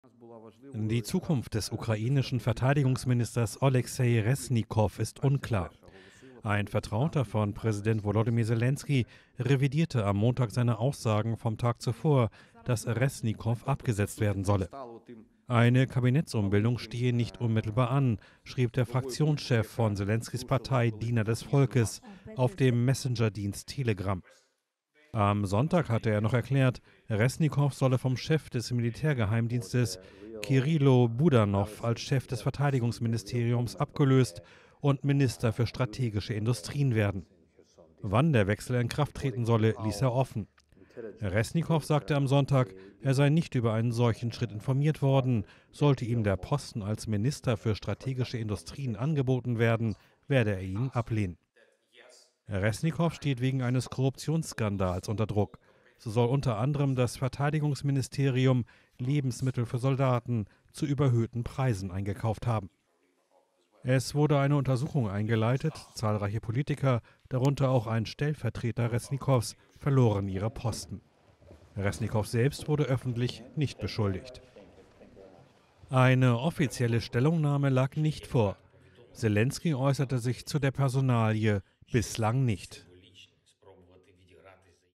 Irritationen um ukrainischen Verteidigungsminister
David Arachamija, ein Vertrauter von Präsident Wolodimir Selenski, hat seine Aussage revidiert, dass Verteidigungsminister Olexij Resnikow aus der Regierung ausscheiden soll. Resnikow steht zur Zeit wegen eines Korruptionsskandals unter Druck.

Alle Informationen zum Krieg in der Ukraine erhalten Sie in unserem News-Blog